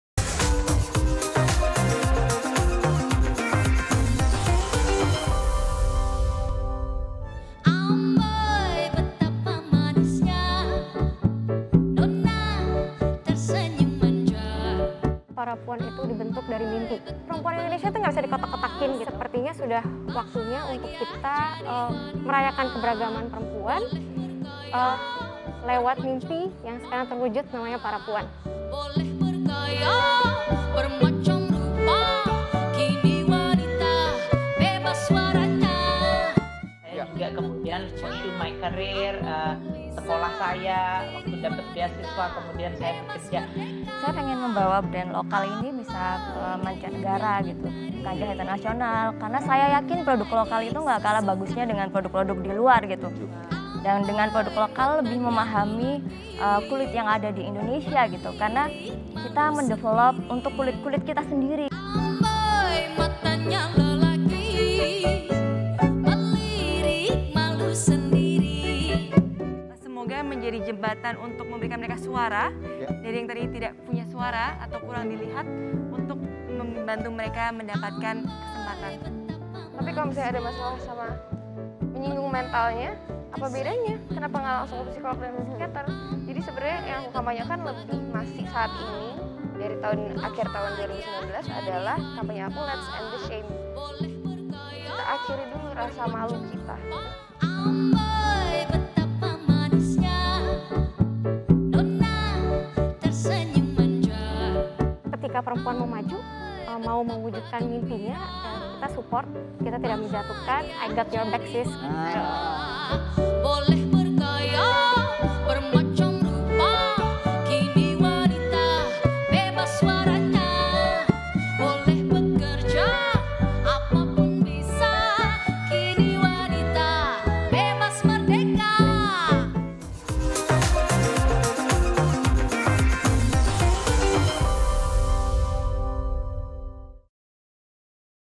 Launching Parapuan (Teaser) - Meraih Mimpi Bersama Parapuan
Perempuan Indonesia memiliki cerita dan perjuangan yang berbeda-beda. Fakta menarik ini menjadi salah satu latar belakang terbentuknya Parapuan yang hadir sebagai ruang aman dan support system bagi perempuan Indonesia dalam meraih mimpinya.

Seperti empat tokoh perempuan yang menginspirasi; Menteri Keuangan RI Sri Mulyani dengan pendidikannya, Shandy Purnamasari dengan produk kecantikannya, Tamara Gondo dengan perjuangannya menciptakan tempat penampungan khusus para pengungsi perempuan, dan Ariel Tatum yang sedang gencar menyuarakan pentingnya kesehatan mental bagi perempuan Indonesia.

Selain Shandy Purnamasari dengan MSGLOW yang merupakan produk lokal, ada juga 4 alat kecantikan berkelanjutan dari brand lokal yang bisa Kawan Puan coba. Berikut penjelasannya!